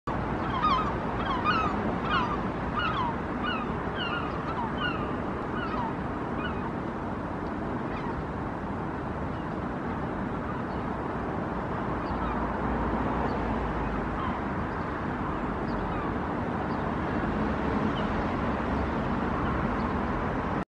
Redshank alert display
This 'bobbing' behaviour is displayed when Redshanks are disturbed and alert. The bird often flies off calling if the disturbance persists. Taken at the river Hull near the Stoneferry Bridges.